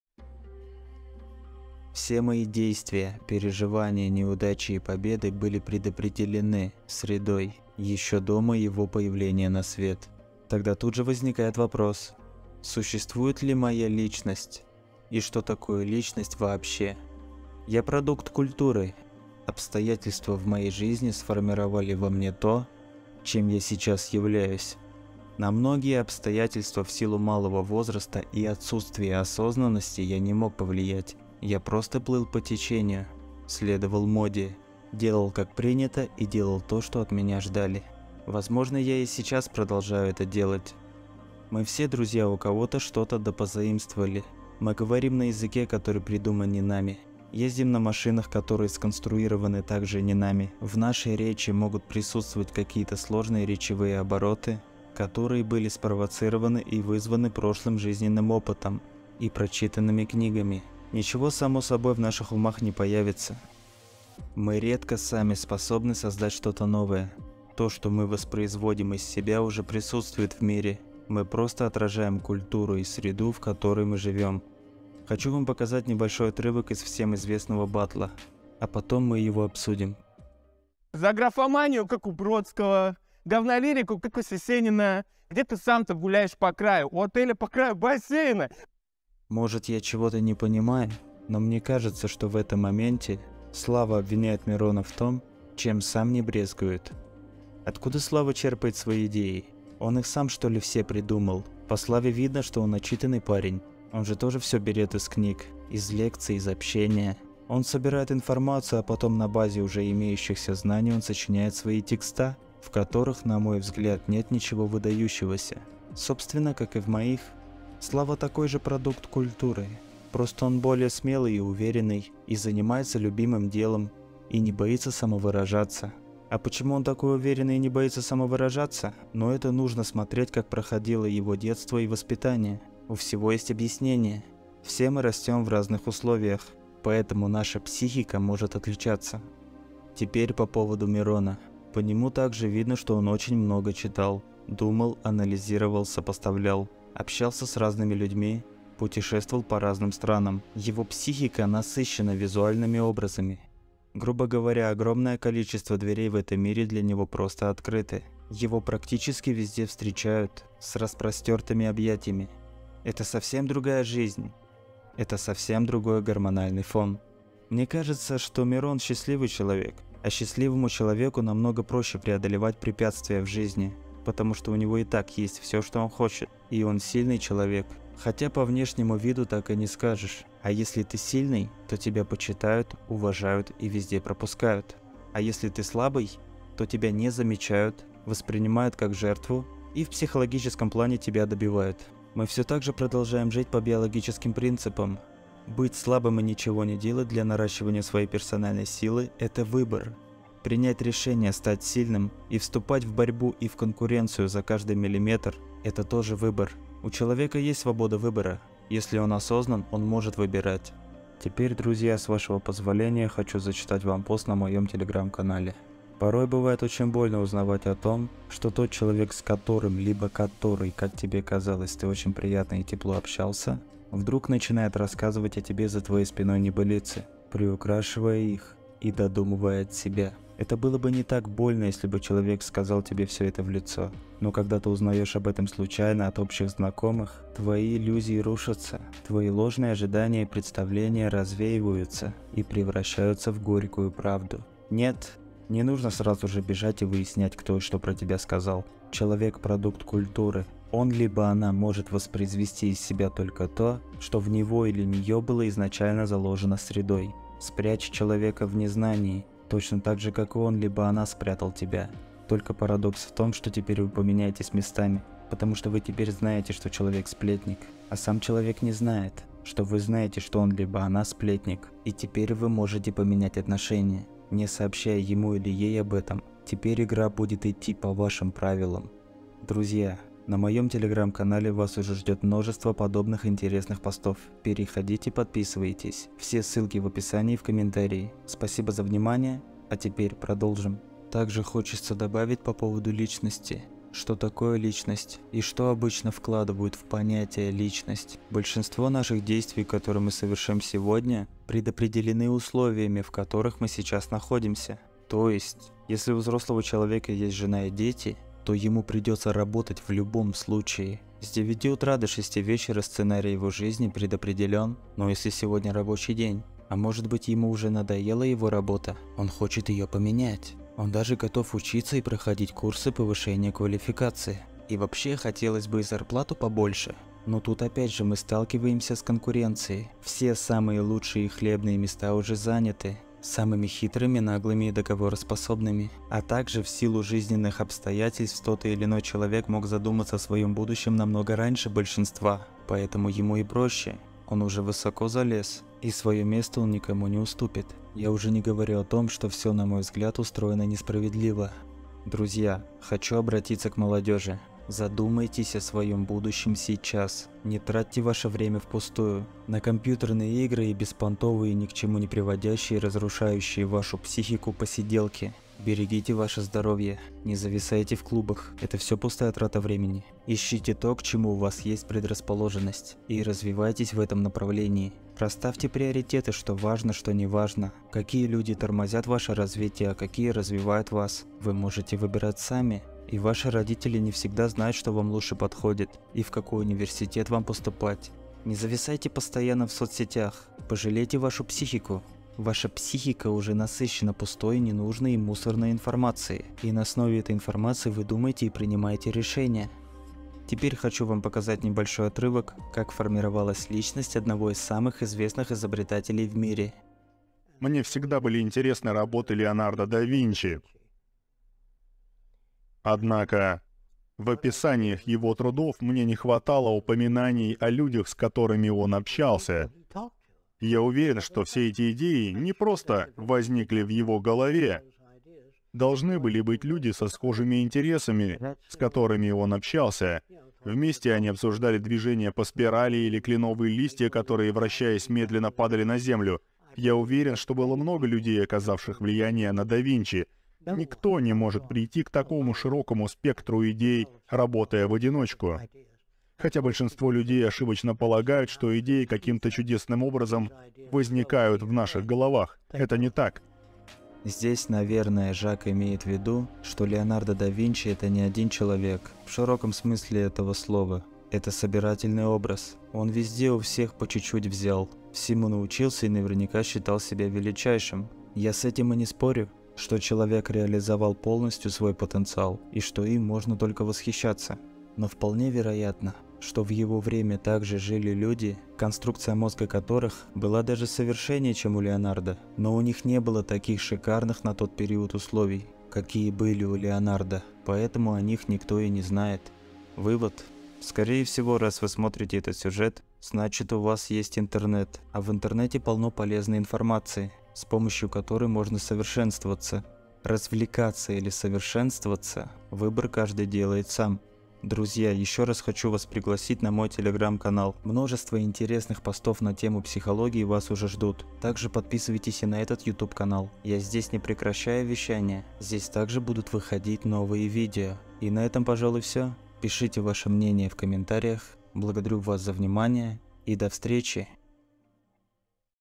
Размышления о существовании личности, на примере Славы КПСС и Оксимирона
Все мои действия, переживания, неудачи и победы, были предопределены средой, ещё до моего появления на свет. Тогда тут же возникает вопрос: существует ли моя личность? И что такое личность вообще?
Я продукт культуры, обстоятельства в моей жизни сформировали во мне то, чем я сейчас являюсь. На многие обстоятельства в силу малого возраста и отсутствия осознанности, я не мог повлиять. Я просто плыл по течению, следовал моде, делал как принято и делал то, что от меня ждали, возможно я и сейчас продолжаю это делать.
В этом видео мы погрузимся в философские размышления о существовании личности. Что такое "Я"? Как мы определяем свою индивидуальность? Может ли личность быть иллюзией? Мы постараемся разобраться в этих вопросах, пройдя через теории философов и психологов, а также рассмотрев некоторые жизненные ситуации. Приготовьтесь к глубокому погружению в свою собственную сущность и выходу за рамки повседневной рутины.

Если у вас есть вопросы и предложения, то пожалуйста, пишите мне в Телеграмм-диалоги